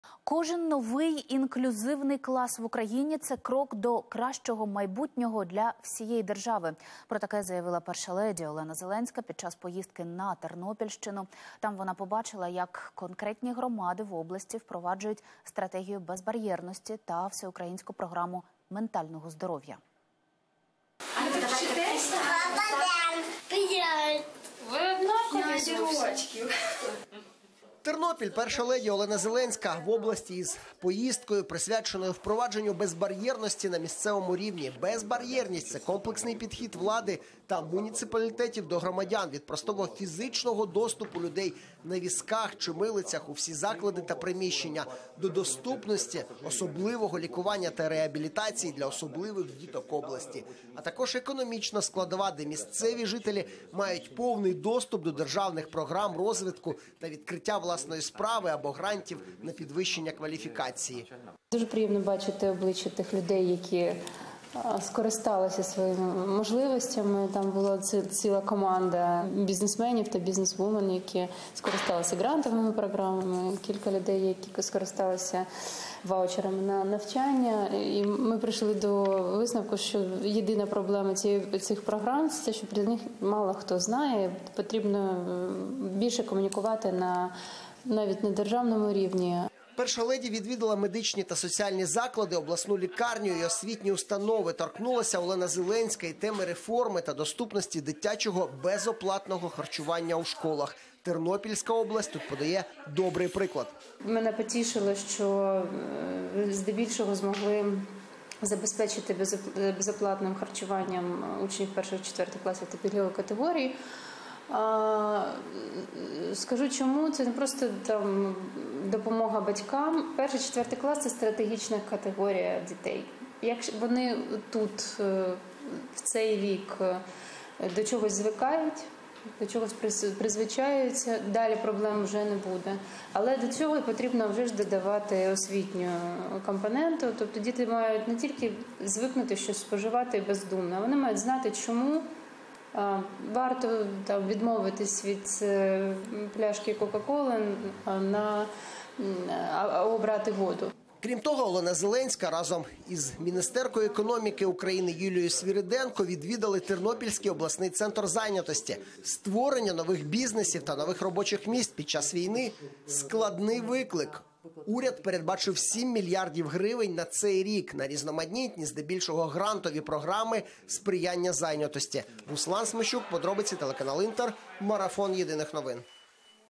Зеленська прибула з візитом на Тернопільщину
Кожен новий інклюзивний клас в Україні - це крок до кращого майбутнього для всієї держави. Про це заявила перша леді Олена Зеленська під час поїздки на Тернопільщину. Там вона побачила, як конкретні громади в області впроваджують Стратегію безбар'єрності та Всеукраїнську програму ментального здоров'я.